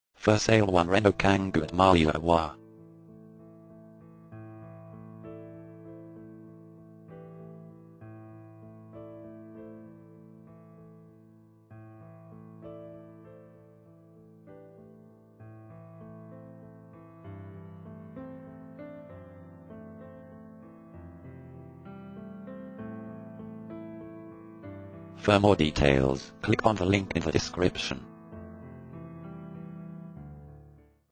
Sale one Renault Kangoo Marly le Roi
For sale at  Marly le Roi  Used car Renault Kangoo of 95000 Km.
used motors Autoenvideo : Sale one Renault Kangoo  Marly le Roi  Yvelines